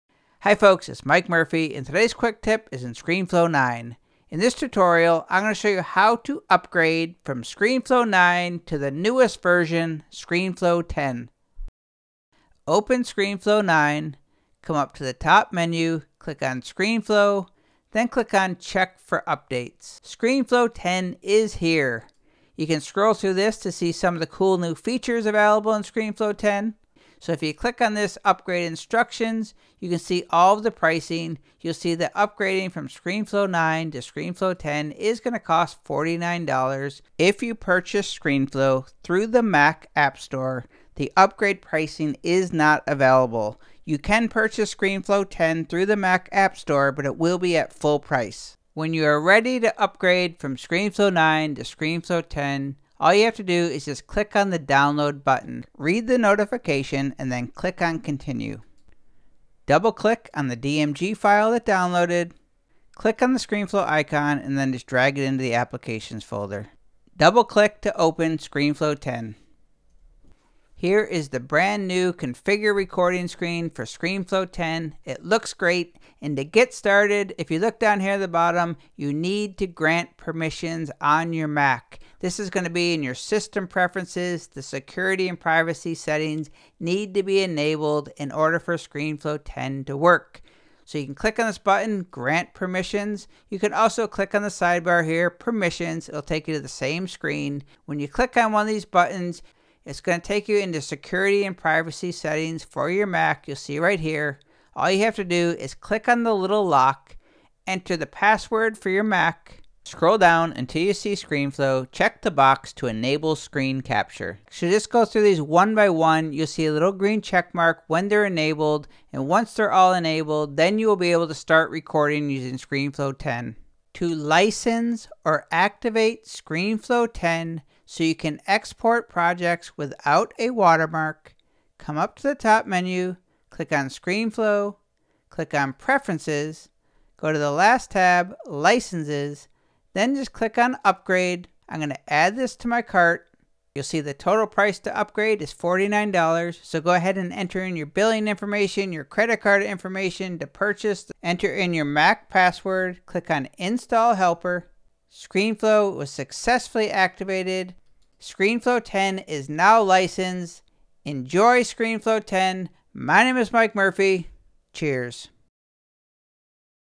Screenflow 10: How To Upgrade from Screenflow 9 (or Older)
This tutorial will show you haw to upgrade from Screenflow 9 to Screenflow 10 and the process is similar if upgrading from an earlier version of Screenflow.

Note: If you purchased Screenflow through Mac App Store, the special upgrade pricing is not available. You will need to purchase Screenflow 10 at full price if you have the Mac App Store version.

How To Upgrade to Screenflow 10 from Screenflow 9:
1. Open Screenflow 9
2. Go to top-left Menu and click on Screenflow
3. Click on ‘Check for Upgrades’
4. Pop-up should appear titled ‘Major Update Available’
5. Click ‘Upgrade Instructions’ to go to Telestream Site
6. Click ‘Download’ to download the .dmg file to computer
7. Double click .dmg file to install
8. Drag icon into Applications Folder
9. Double-click Screenflow 10 icon to open

How to Grant Permissions:
1. Click on Permissions in the sidebar of Screenflow 10 Configure Recording Screen that pops up when you first open Screenflow 10
2. Click on ‘Enable Screen Capture’
3. System Preferences…Security & Privacy will open
4. Double-click on the lock in the bottom left corner
5. Enter Mac password
6. Scroll in the App list and check the box for Screenflow
7. Repeat to Enable Camera & Microphone

How To Activate Screenflow 10 (Exports will have watermark until you purchase the upgrade)
1. Click on Screenflow in the top menu
2. Click on ‘Preferences’
3. Click on ‘Licenses’
4. Click on ‘Upgrade’
5. Click on ‘Add to Cart’
6. Enter credit card info
7. Pay $49 upgrade fee
8. Email confirmation will contain Serial Number if it does not auto-populate
9. Follow prompts until you get Success Message and see ‘Screenflow is Licensed’

How Find the Serial Number in Screenflow 9:
1. Open Screenflow
2. Go to top menu and click on Screenflow
3. Click on Preferences (or use keyboard shortcut cmd + comma)
4. Click on ‘Licenses’
5. Triple-click in the field to highlight the license key
6. Copy (cmd + c) if needed